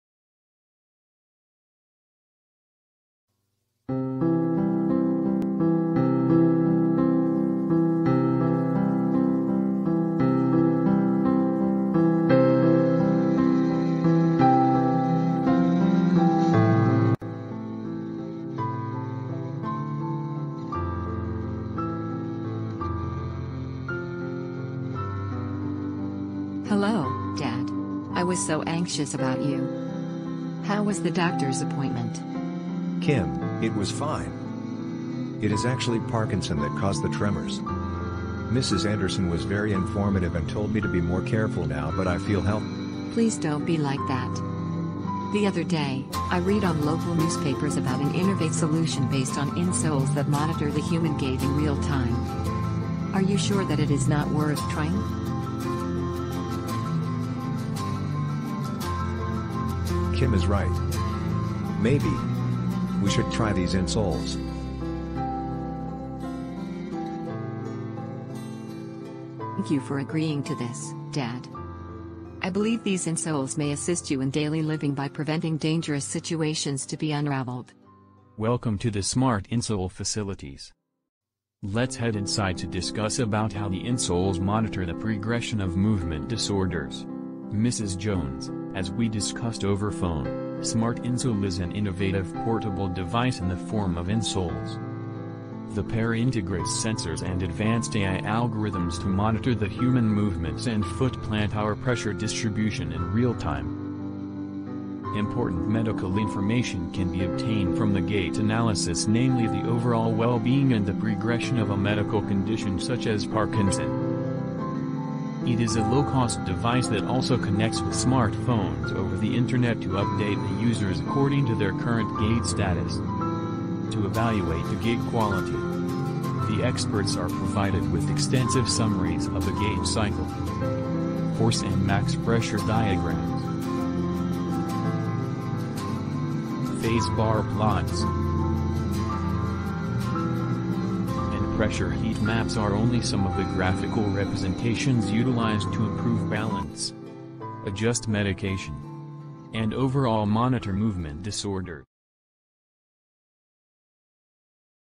The Smart Insole Project
The basic idea of the project is to design and manufacture a smart, wearable insole with built-in pressure measurement sensors, other micro-electronic sensing elements and communication devices to tackle the challenge of efficient gait monitoring in real life. This wearable insole is based on a flexible piezoelectriclayer, with a totally different architecture compared to currently internationally available technologies.
The main innovative features of the project are: a) the development of a new wearable based on a novel architecture, as compared to existing approaches, in terms of sensitivity and cost; b) the development of an advanced spatiotemporal signal processing algorithms; and c) the development and application of innovative machine learning methods for accurate analysis of gait and its morphology. These innovations will be the basis for designing new services for the early diagnosis of specific pathological conditions, monitoring their development and evaluating the use of wearable sensor platforms as clinical diagnostic tools.